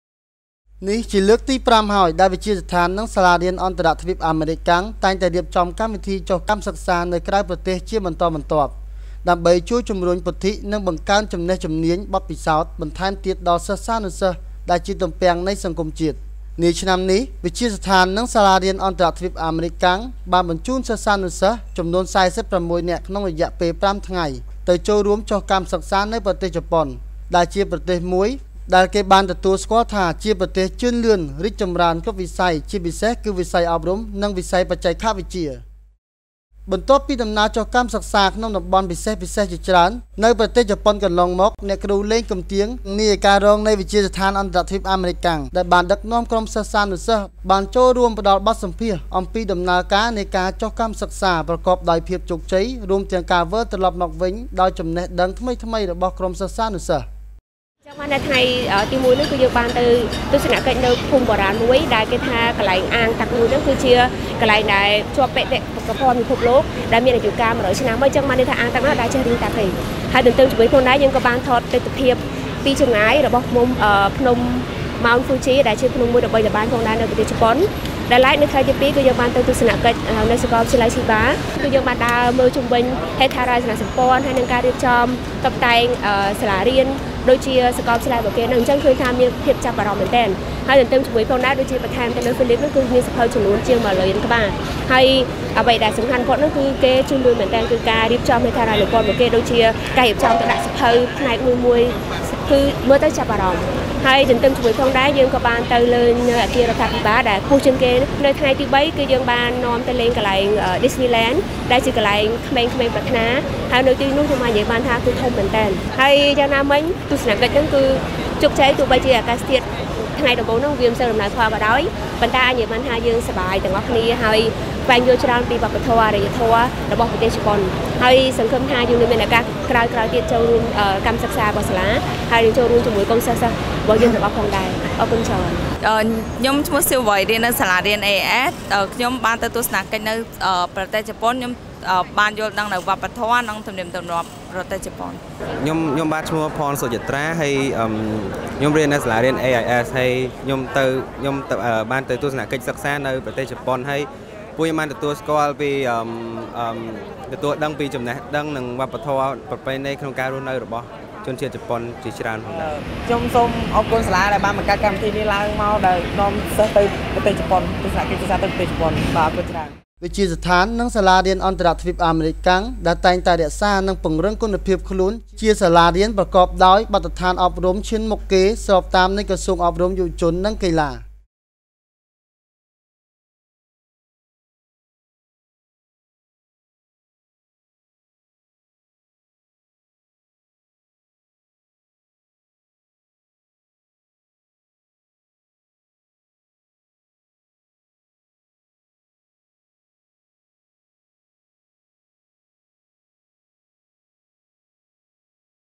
ដំណើរទស្សនកិច្ចសិក្សាលើកទី៥ ទៅកាន់ប្រទេសជប៉ុន
Mengly J. Quach Production
MJQ Production

20170223 - សិស្សានុសិស្សវិទ្យាស្ថាន និងសាលារៀន អន្ដរទ្វីប អាមេរិកាំង ដំណើរទស្សនកិច្ចសិក្សាលើកទី៥ ទៅកាន់ប្រទេសជប៉ុន